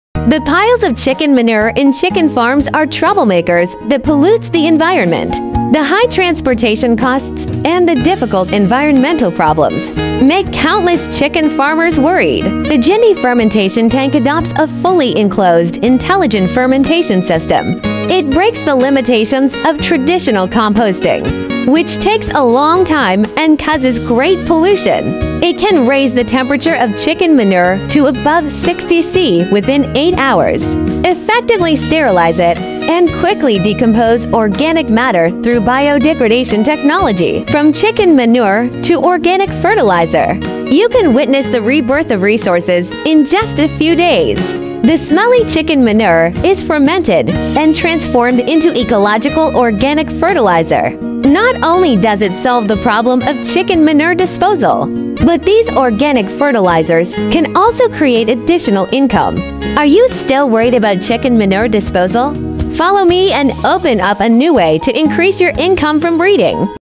Fermentation tank turn chickenmanure into organic fertilizer
The Jindi fermentation tank adopts a fully enclosed intelligent fermentation system. It breaks the limitations of traditional composting, which takes a long time and causes great pollution. It can raise the temperature of chicken manure to above 60℃ within 8 hours, effectively sterilize it, and quickly decompose organic matter through biodegradation technology.
From chicken manure to organic fertilizer, you can witness the rebirth of resources in just a few days! The smelly chicken manure is fermented and transformed into ecological organic fertilizer. Not only does it solve the problem of chicken manure disposal, but these organic fertilizers can also create additional income. Are you still worried about chicken manure disposal? Follow me and open up a new way to increase your income from breeding!@CompostingTankJinDi